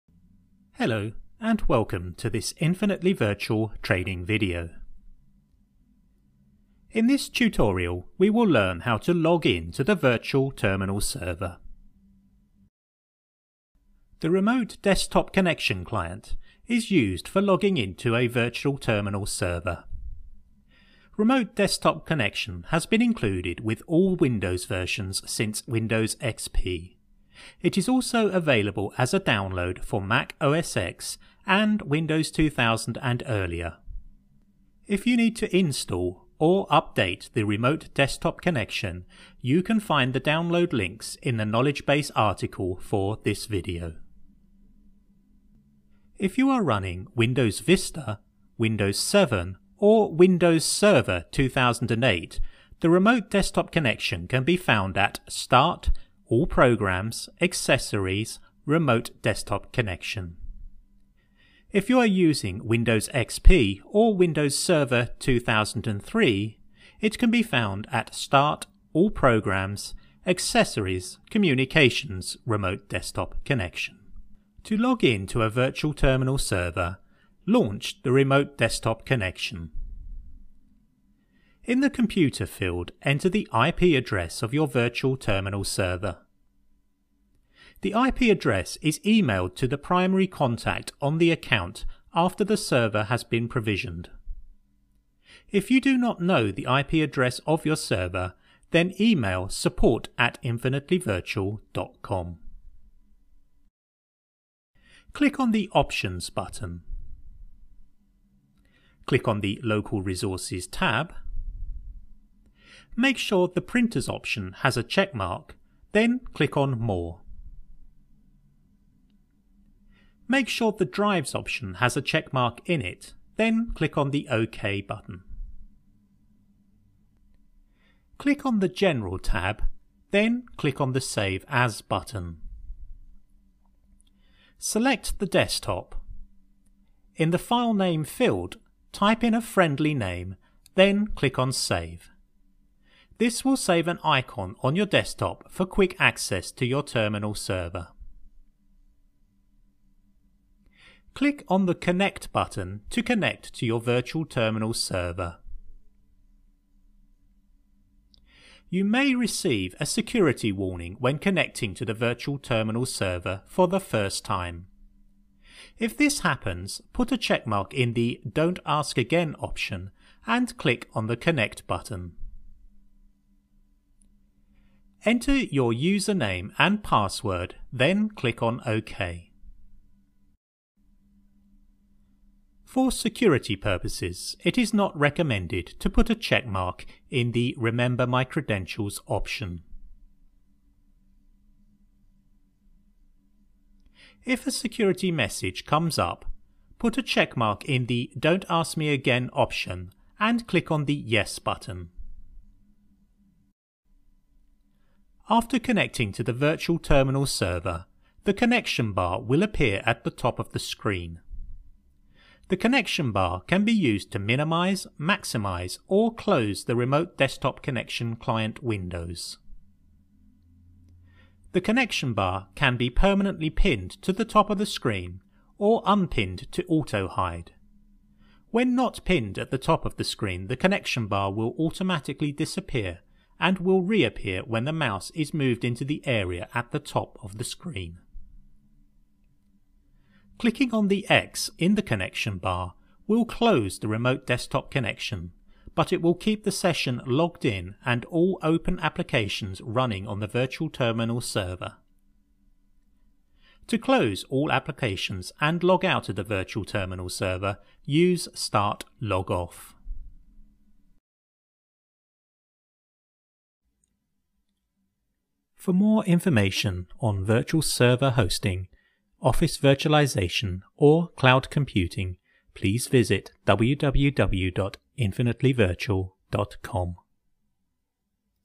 How to Login to a Terminal Server with Remote Desktop Client
This video demonstrates how to login to a terminal server with Windows Remote Desktop Client.